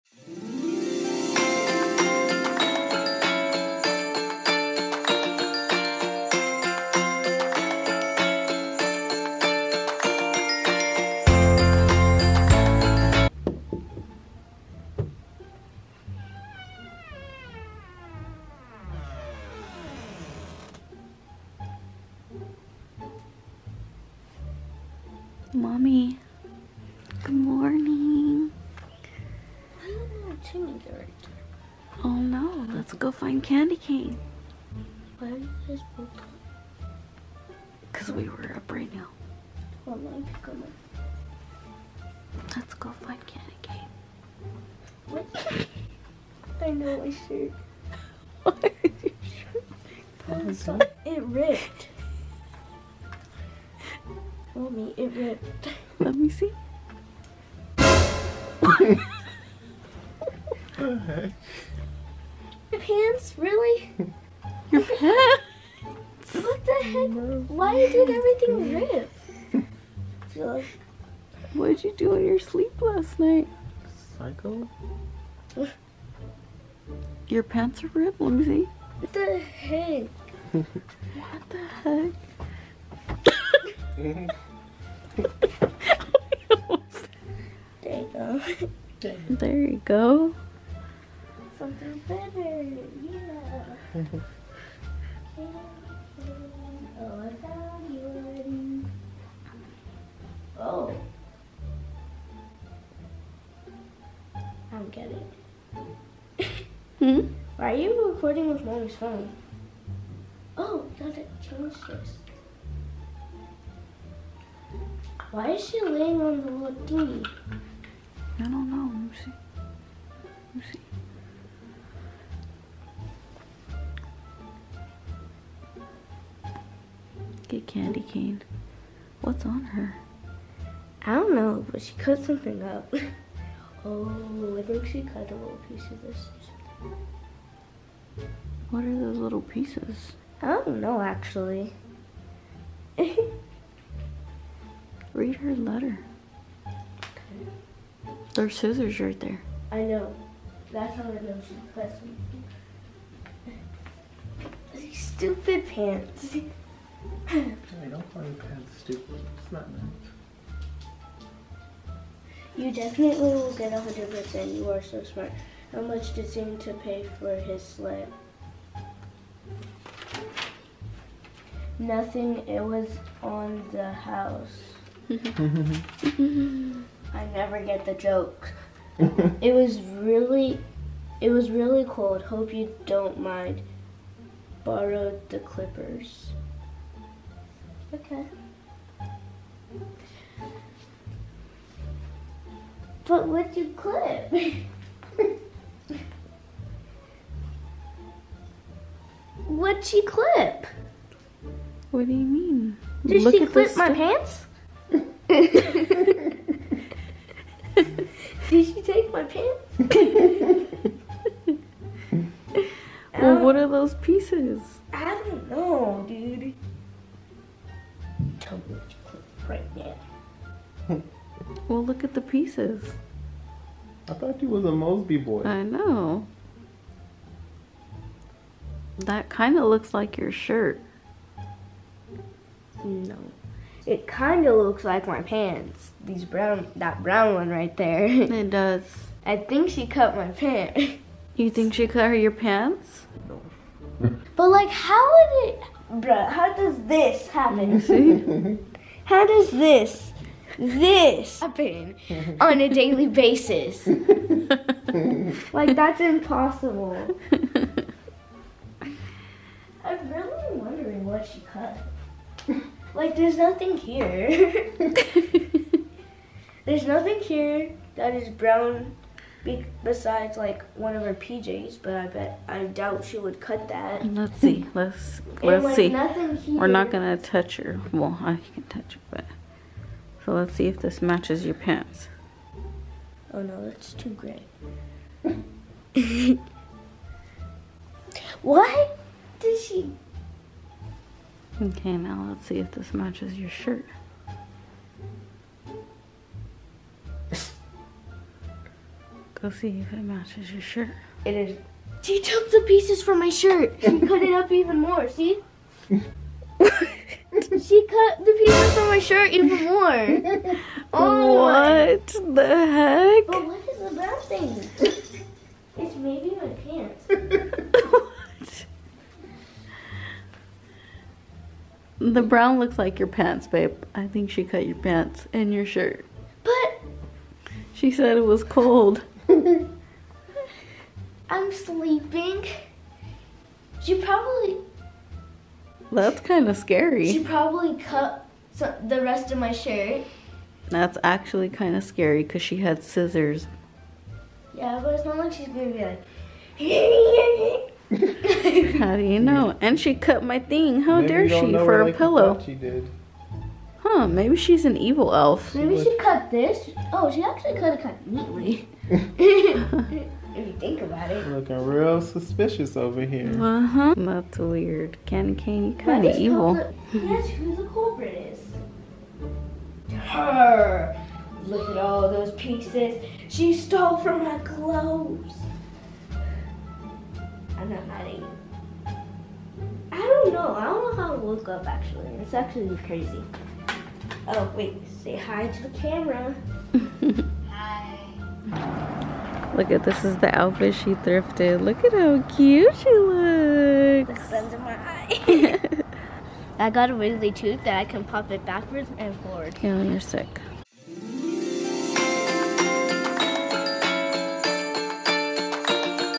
HER ELF ON THE SHELF PRANKED HER! *HILARIOUS*
HER ELF ON THE SHELF PRANKED HER THIS MORNING AND CUT HER PAJAMAS WITH SCISSORS!